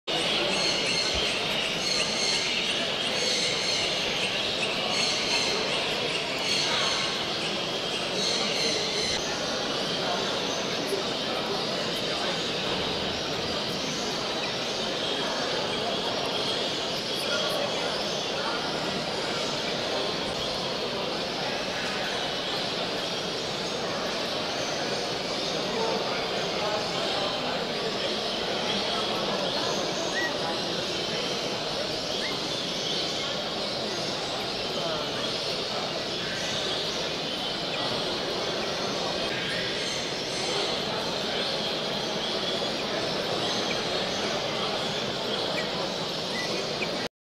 Deutsche Meisterschaft 2018 DKB - Vogelschau. Dompfaff Braun Gelb.
69.Deutsche Meisterschaft 2018 des Deutschen Kanarien- und Vogelzüchter Bundes e.V. (DKB) am 06. und 07.Januar 2018.